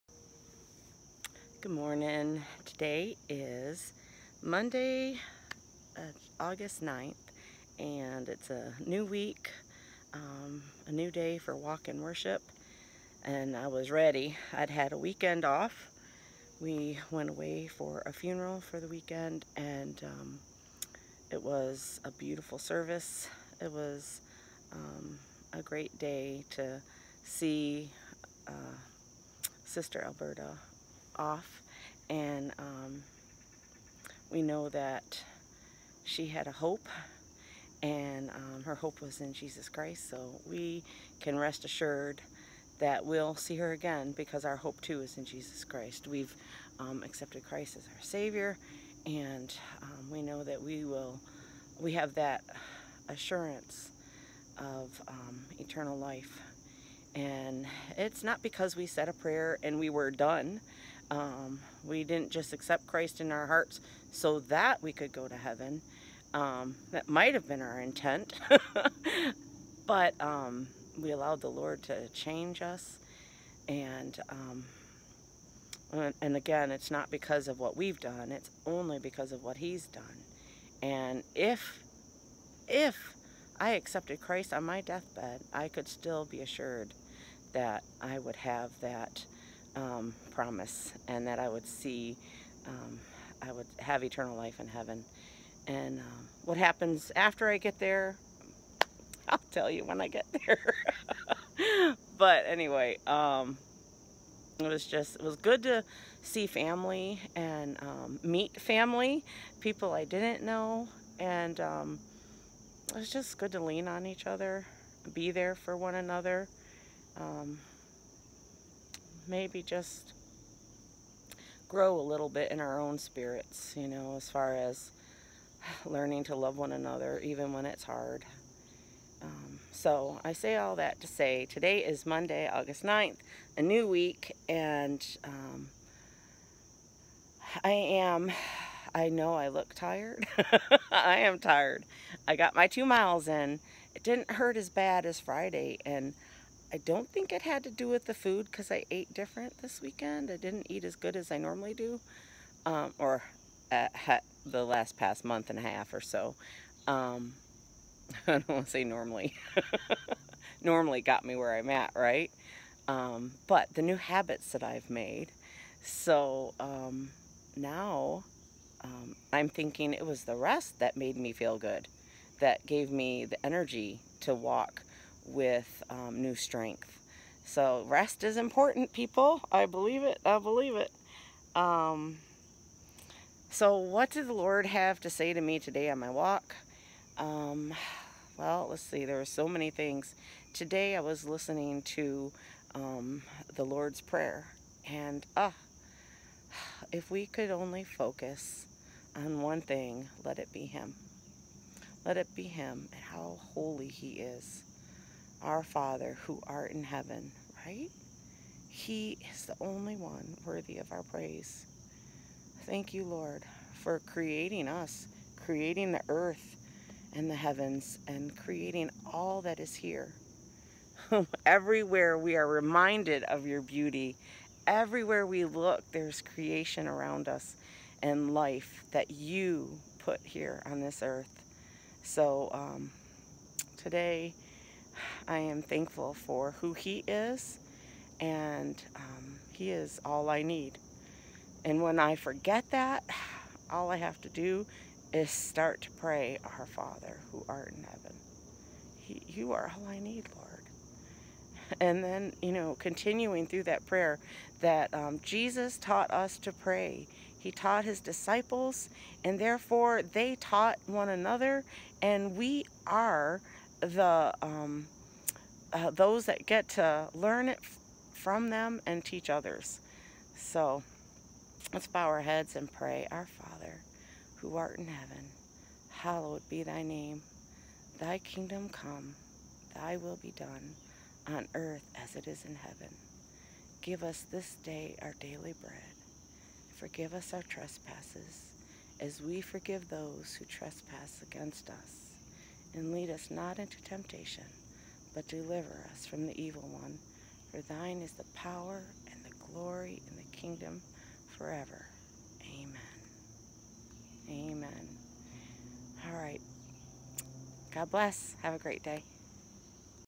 Walk and Worship 8-9-21
Saying goodbye to Aunt Alberta, knowing the hope she had in Christ Jesus and how we can see God’s creation all around us! The Lord’s prayer will renew our strength! ❤️🙏

  9  Our Father which art in heaven, Hallowed be thy name.
10 Thy kingdom come, Thy will be done in earth, as it is in heaven.
11 Give us this day our daily bread.
12 And forgive us our debts, as we forgive our debtors.
13 And lead us not into temptation, but deliver us from evil: For thine is the kingdom, and the power, and the glory, for ever. Amen.